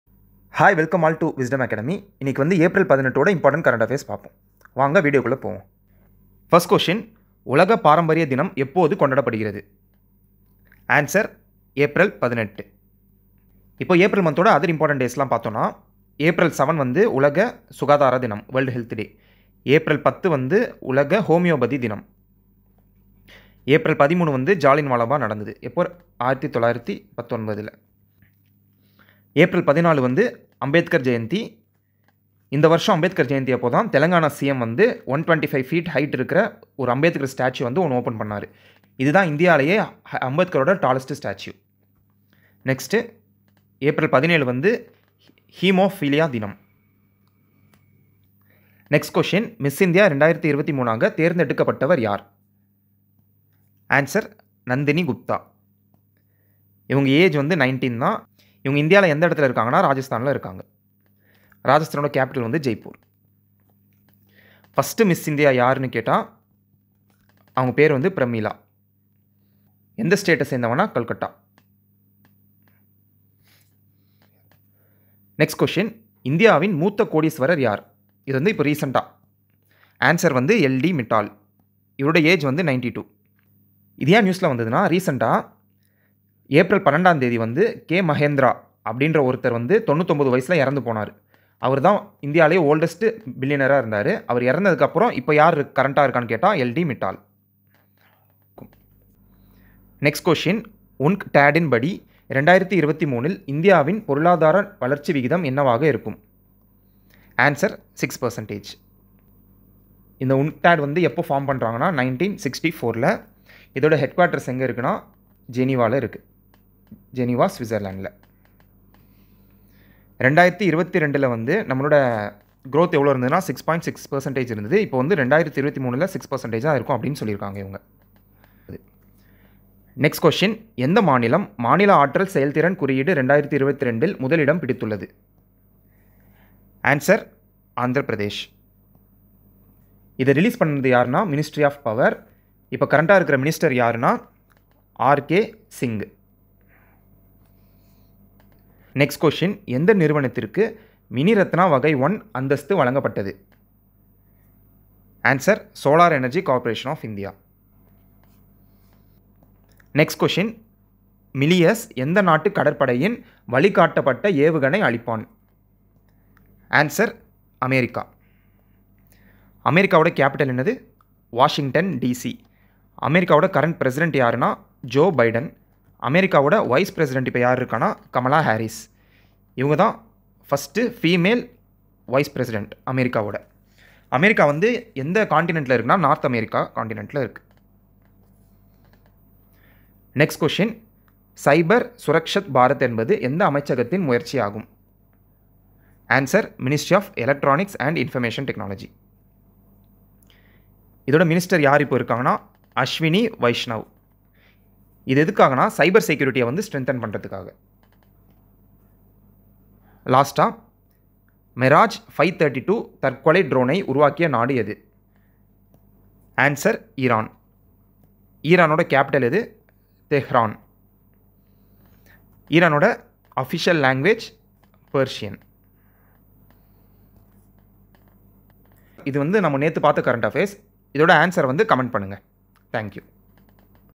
📰⚡18 April 2023 | முக்கிய நடப்பு நிகழ்வுகள் | Daily Current affairs in Tamil and English
- The Wisdom Academy Presents Daily Current affairs in Tamil and English in MCQ format
- PDF can be downloaded from our telegram channel